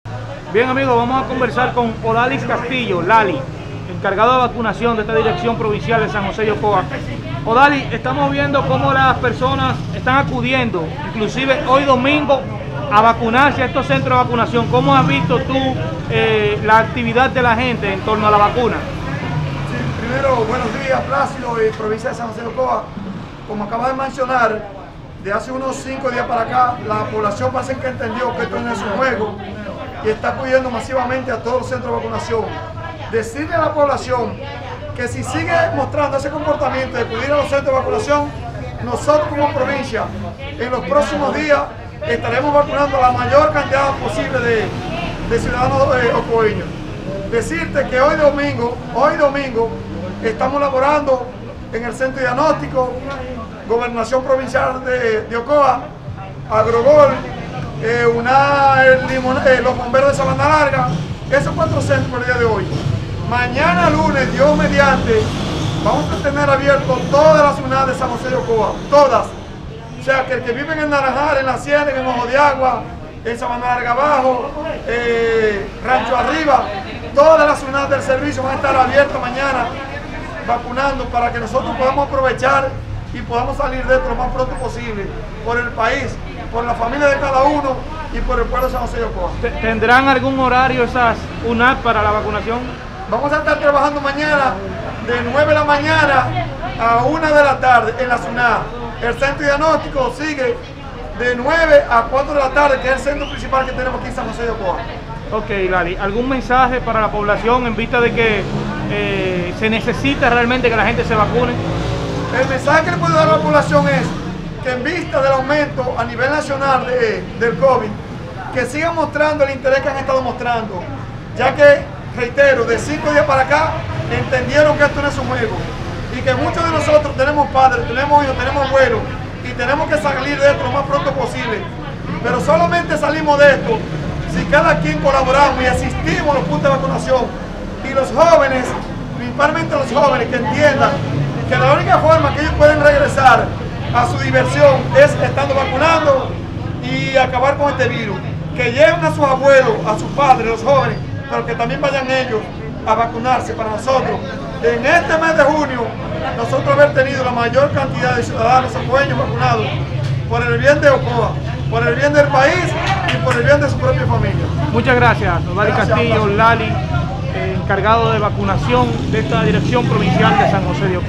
LALY CASTILLO DIRECTOR DE VACUNACIÓN: “DESDE ESTE LUNES 7 EN TODAS LAS UNAPS ESTAREMOS VACUNANDO”
Bienvenidos a " PLACIDO TE INFORMA " disfruta de entrevistas, reportajes, informaciones, comentarios y opiniones. Todo sobre el acontecer noticioso de San José de Ocoa esta aquí !